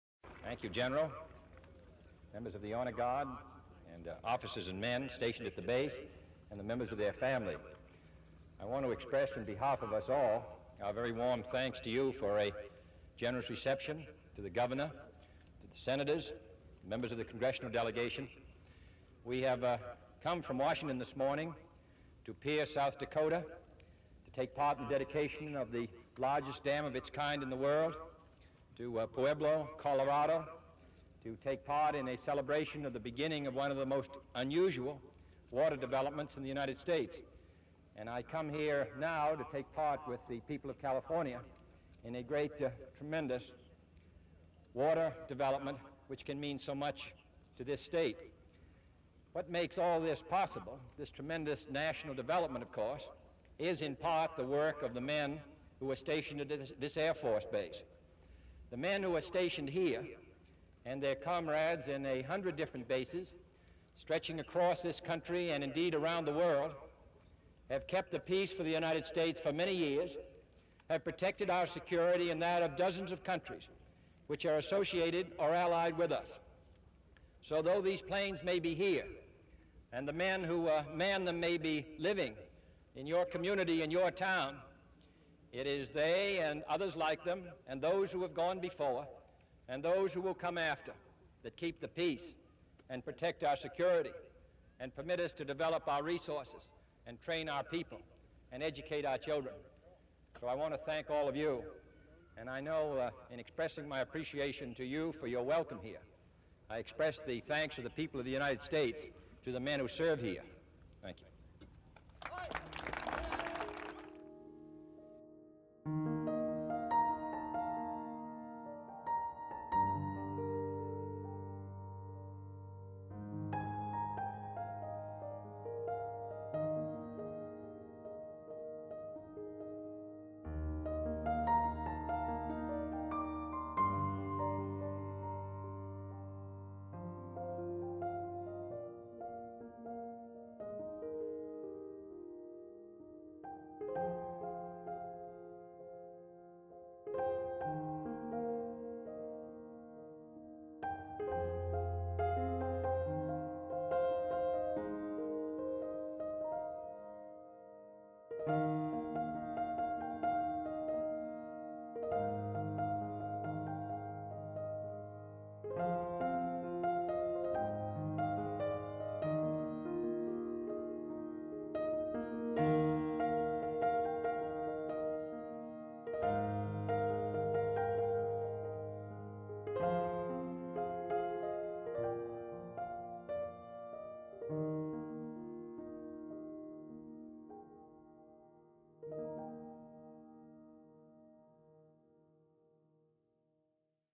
August 17, 1962 - President John F. Kennedy’s remarks upon his arrival at Castle Air Force Base, CA
President John F. Kennedy and his party arrives at Castle Air Force Base and are greeted by Brig. General William R. Yancey, Colonel James V. Reardon and Colonel Leonard S. Dysinger. Full military honors are rendered the President and he reviews the troops. The President makes a brief address and then departs by helicopter and flies to Yosemite National Park, where he arrives 30 minutes later.